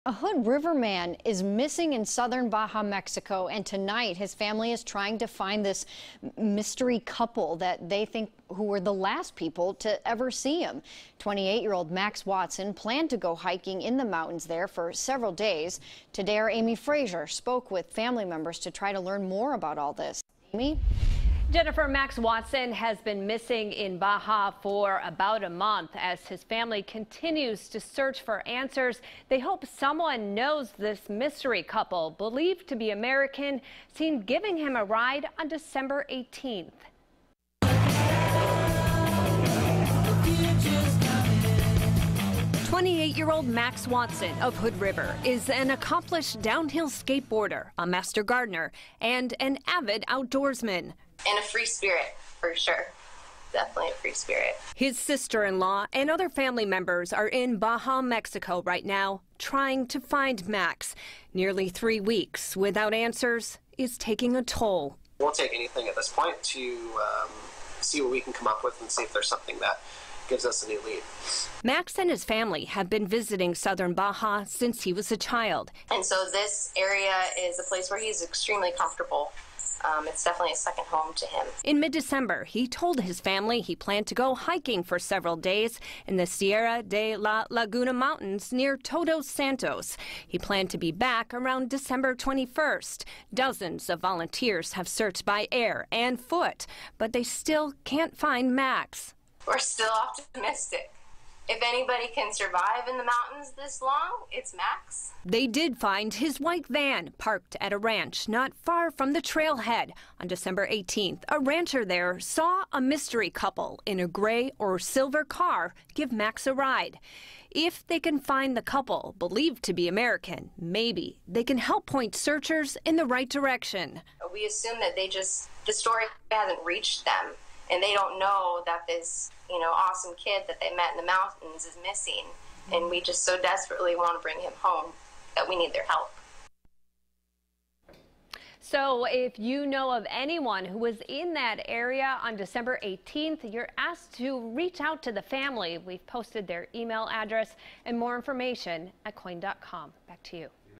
Hood River man missing in Baja; mystery 'gringos' sought
Max Watson has not been seen since December 18, 2018. Since that time, more than 100 US and Mexican volunteers have searched on the ground and by air to find him